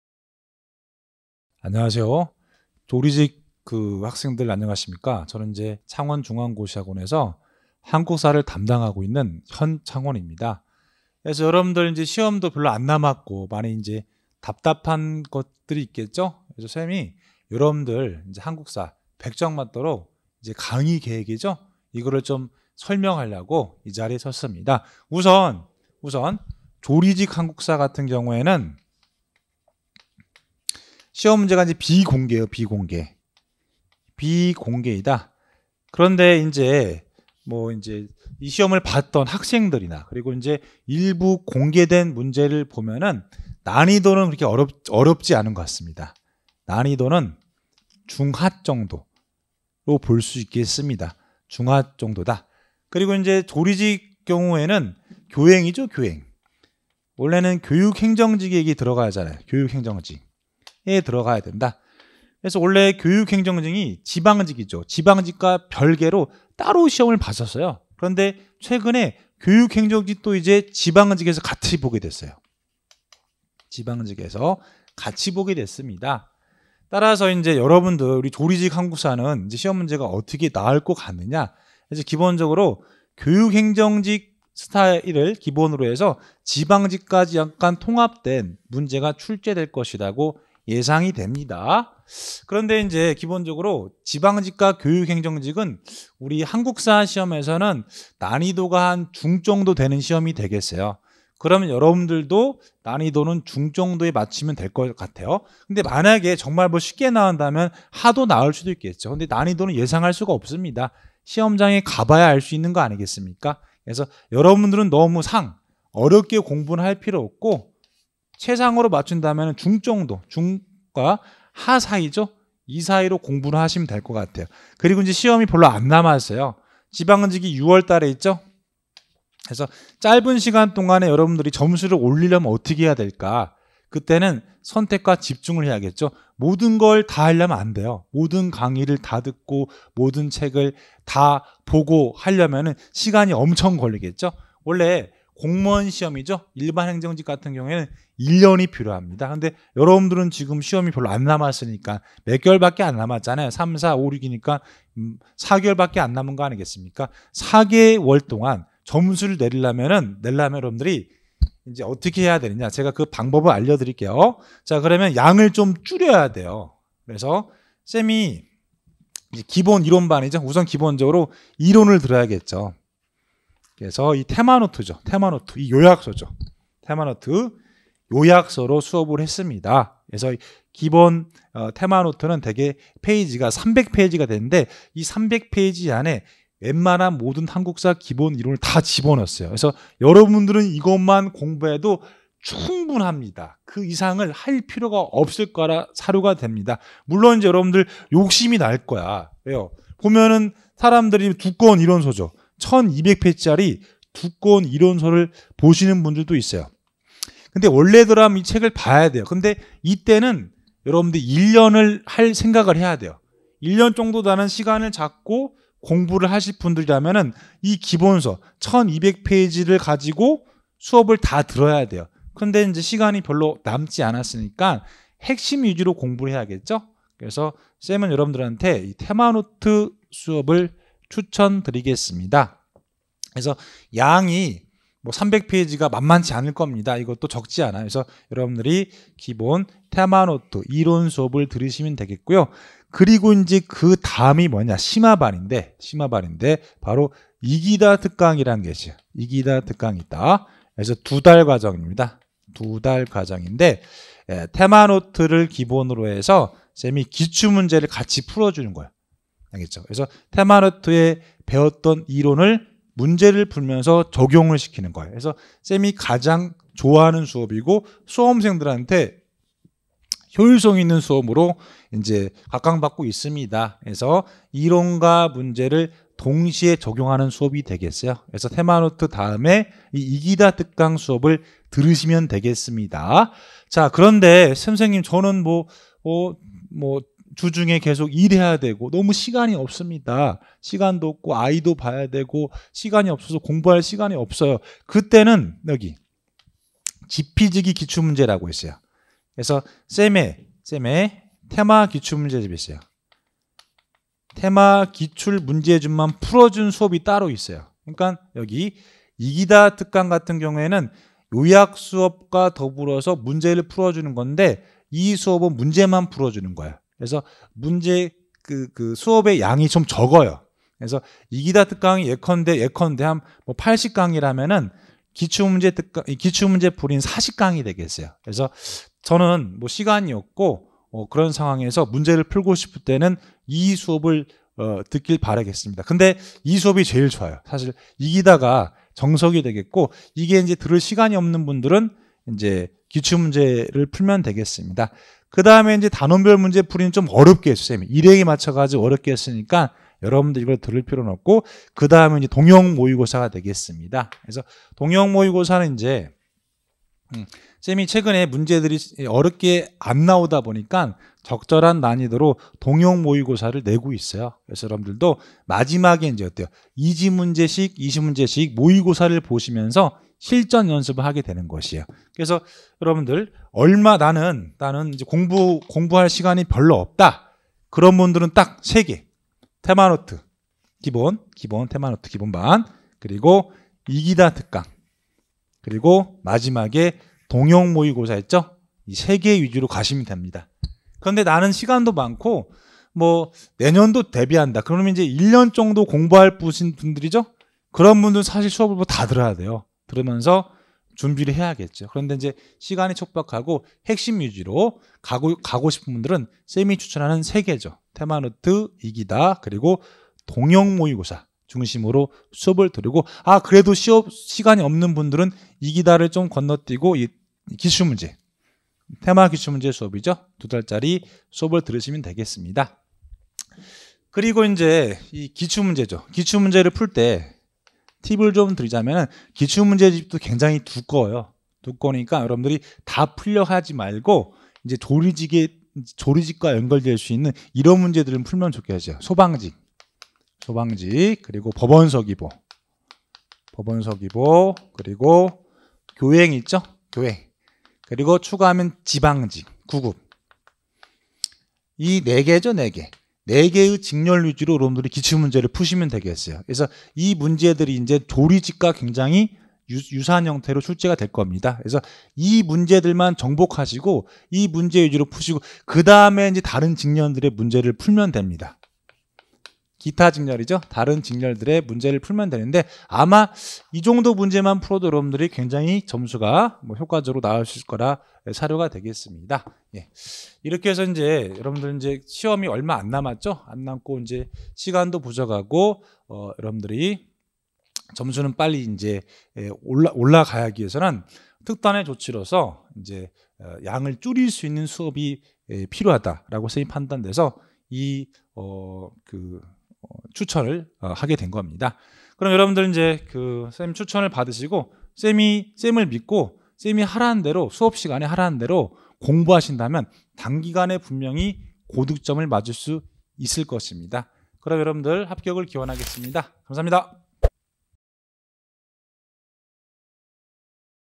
9급조리직공무원 시험 한국사 공부전략! 와우에듀 현창원 필합 전략!
[와우에듀] 현창원 조리직 한국사 공부전략!

방대한 분량에 결코 만만치않은 한국사 공부!
와우에듀의 현창원 한국사선생님이 해결해 드립니다!
조리직 공무원 한국사 시험의 궁금증을 해결해 드립니다!
9급조리직공무원 수험생의 합격위한 공부전략!

현창원선생님 강의는 와우에듀에서 수강할 수 있습니다.